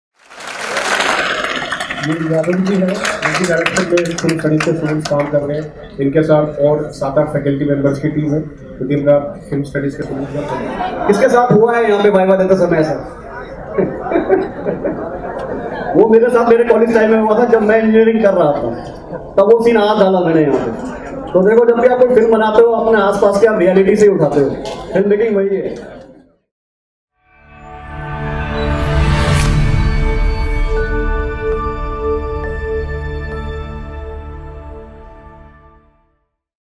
Talking about film making & sharing their experience @ CMFF2 | Chandigarh University
Directors talking about film making & sharing their  experience at 2nd Chandigarh Music And  Film Festival 2022 | Chandigarh University